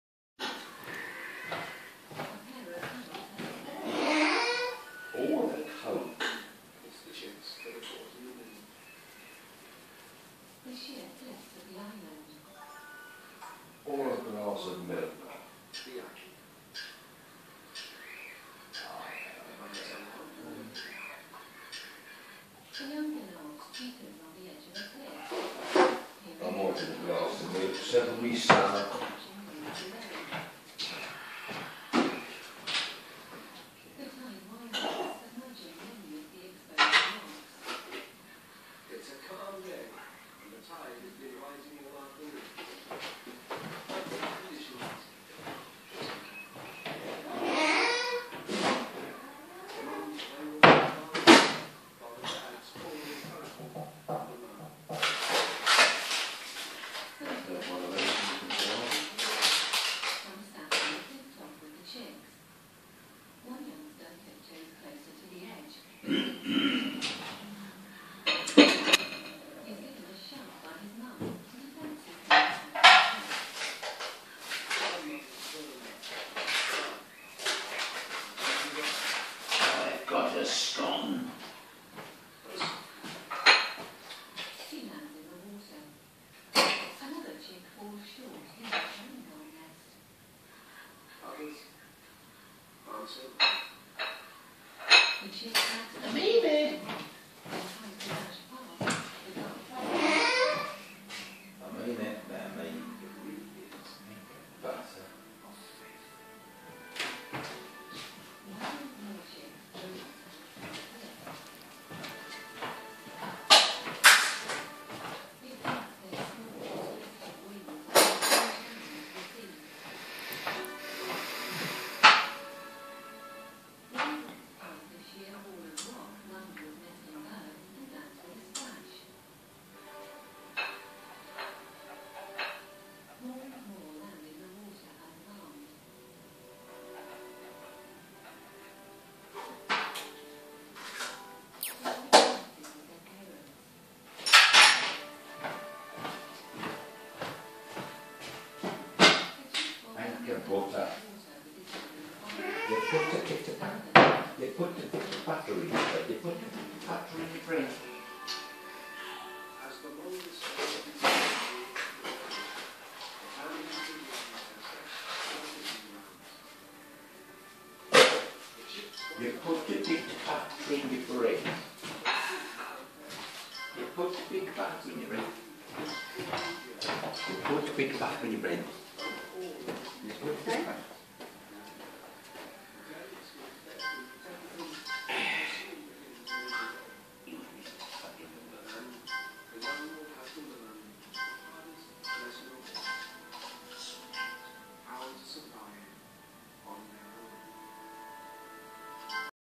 My dad just now making his sandwich and talking to himself when making a sandwich